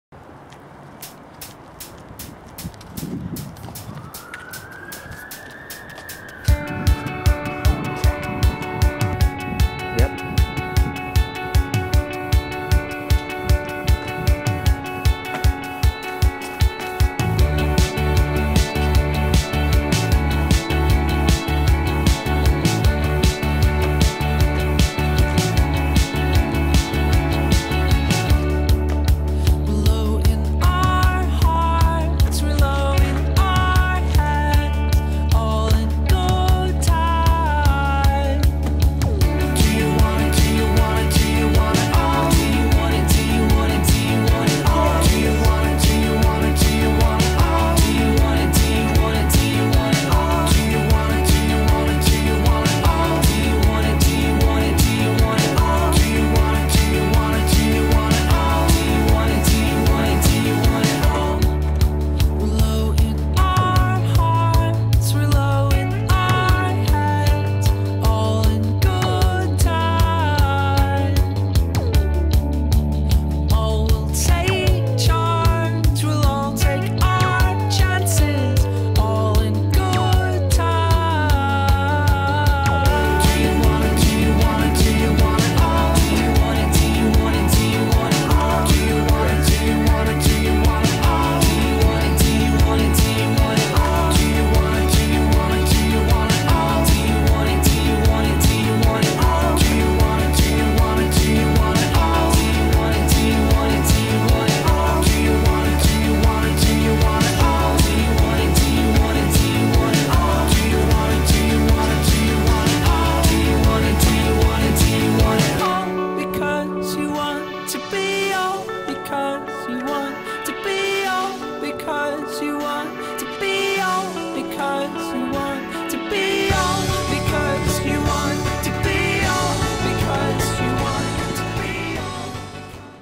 Toge and Drift take on the Hammersmith Ring of Fire
Panos and I spent a few hours exploring a load of new spots in Hammersmith as found by Andy Pearson. Nothing special - just simple movement.